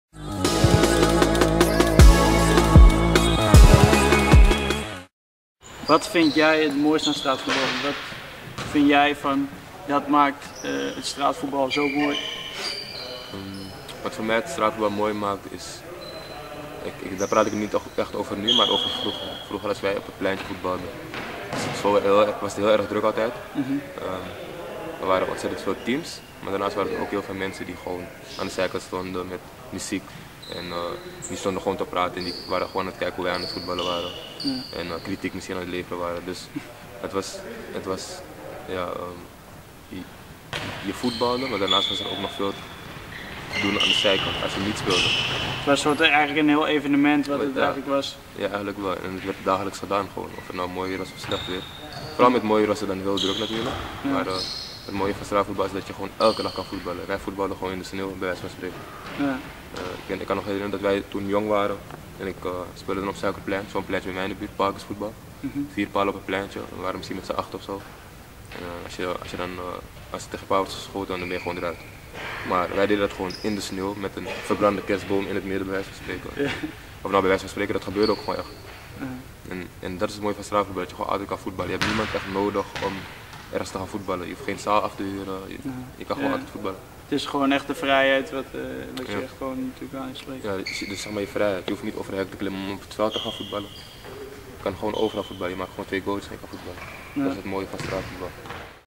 Skills Only - You can Always play It
Orry 'Klapper' Stoc Promes talkes about the most beautiful thing about street soccer. Enjoy. Skills Only